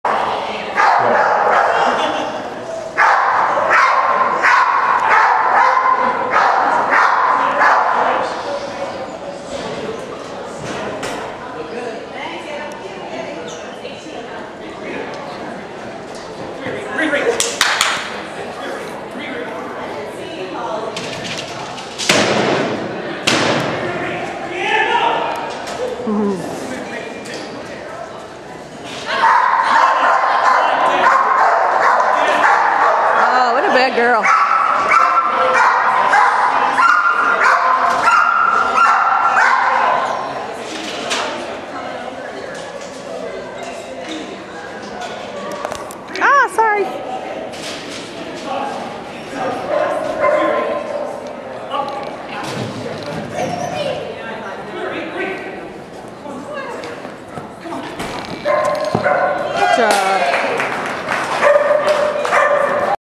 Rhian's MX Run
Montage Works Like A Charm MX MXJ (just turned 3). Here is Rhian's MX run...check out the sassy girl at the table...that's gonna cost us some time!